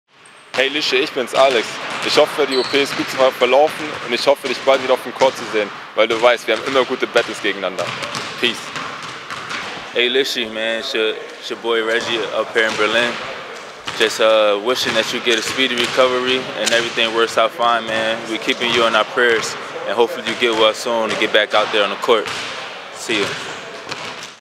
Berliner Genesungs-Wünsche für Johannes Lischka
Die ALBA-Spieler Alex King und Reggie Redding übermitteln per Video ihre besten Wünsche Johannes Lischka nach seiner Tumor-OP.